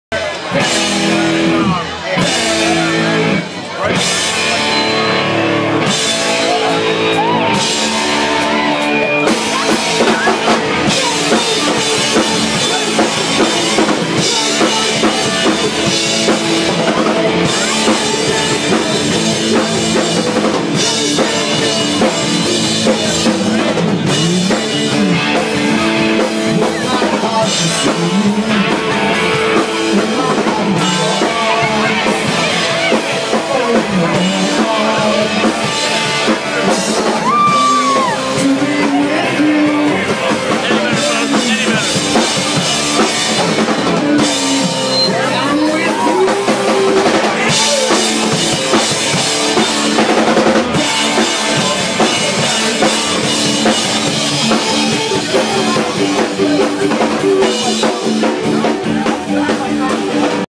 Eddie Vetter live at Stanley's
eddie popped in for Sunday night karaoke!